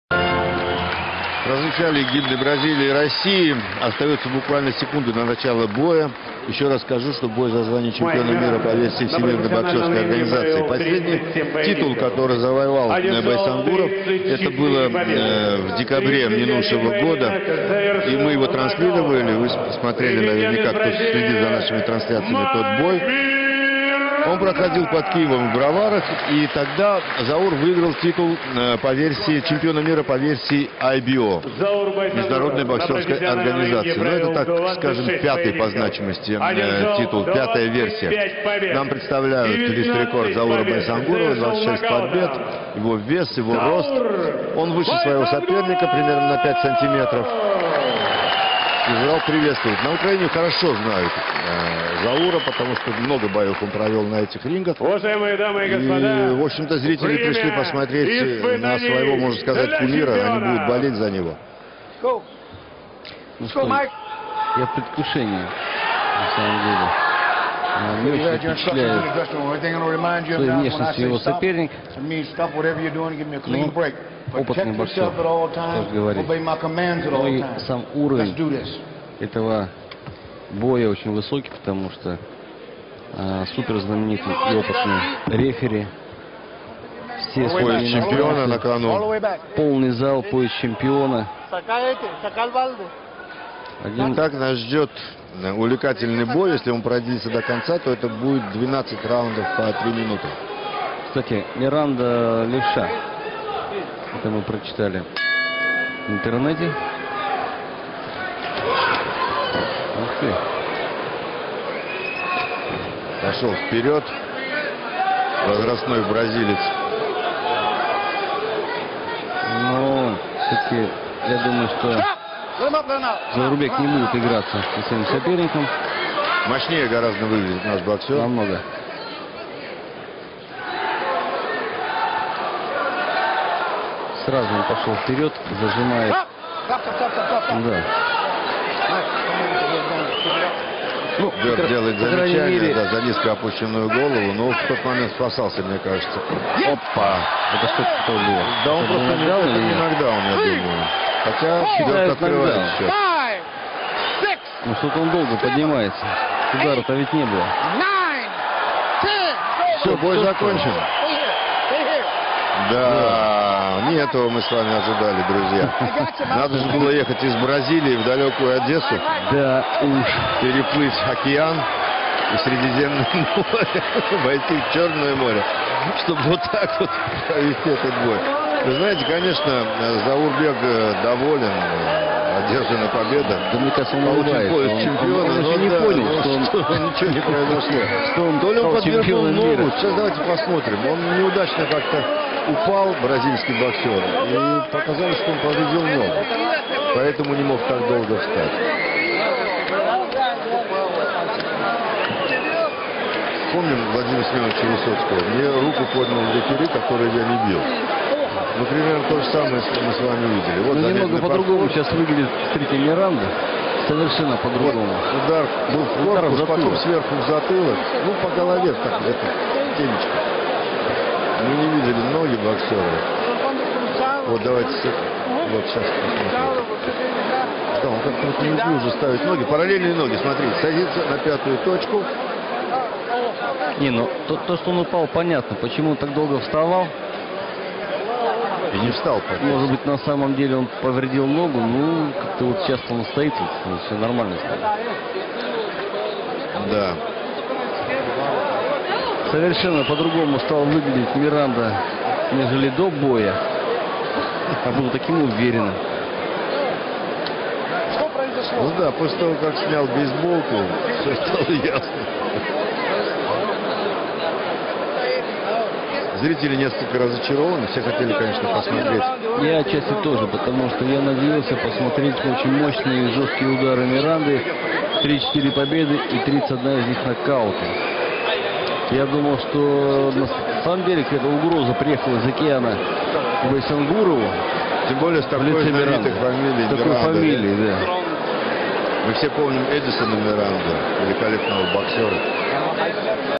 lamentable pelea entre Baysangurov y Miranda
Se disputaba el título mundial interino superwelter de la OMB. Pero al brasileño Mike Miranda no pareció importarle la trascendencia del título: se dejó caer a los 40 segundos y leregaloel triunfo al ruso Baysangurov